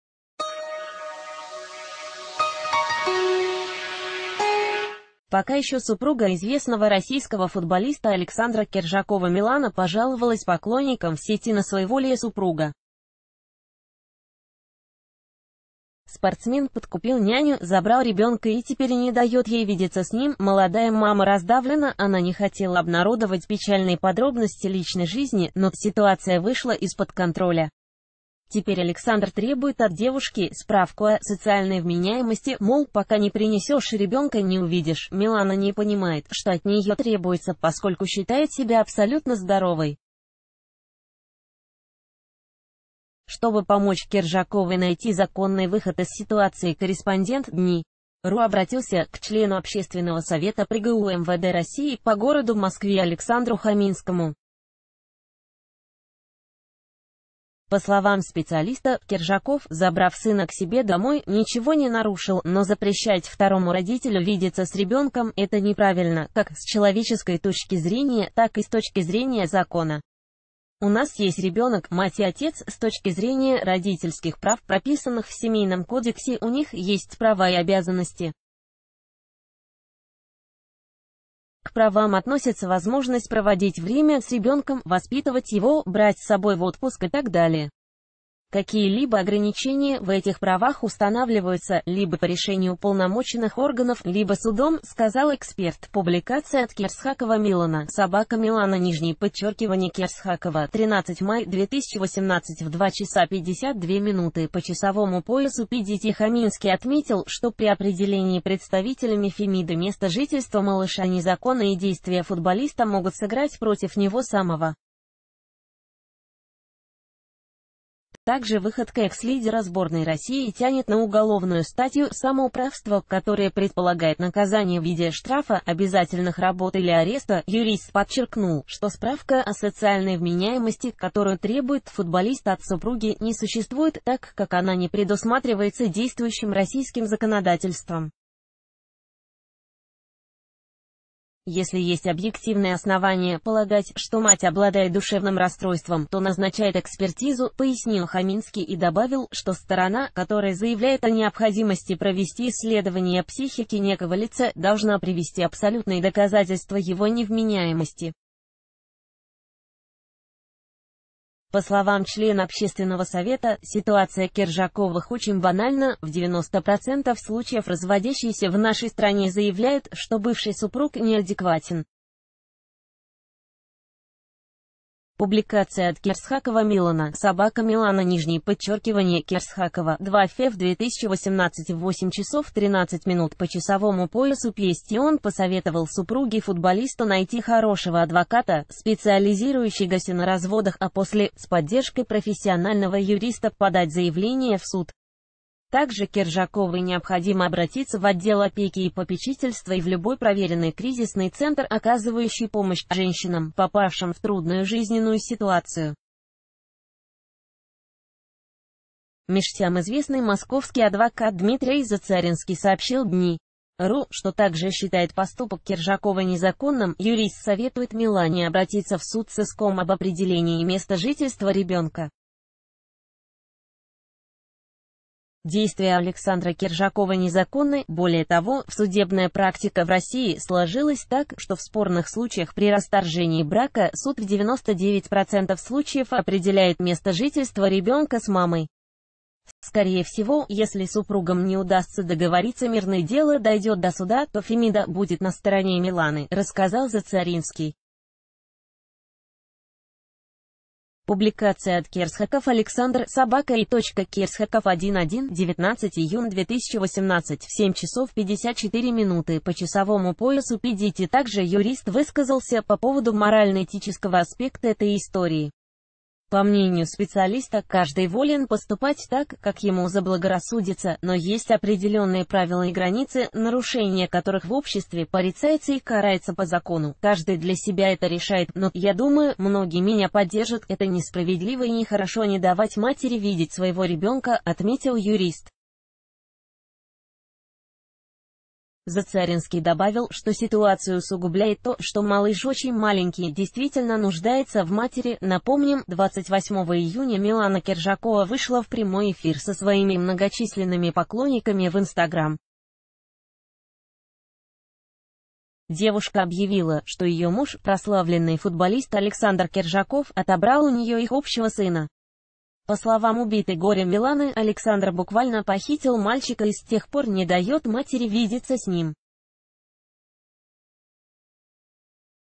Кержакова ждет суровое наказание за похищение ребенка | TVRu
Пока еще супруга известного российского футболиста Александра Кержакова Милана пожаловалась поклонникам в Сети на своеволие супруга. Спортсмен подкупил няню, забрал ребенка и теперь не дает ей видеться с ним.  Молодая мама раздавлена, она не хотела обнародовать печальные подробности личной жизни, но "ситуация вышла из-под контроля". Теперь Александр требует от девушки справку о "социальной вменяем...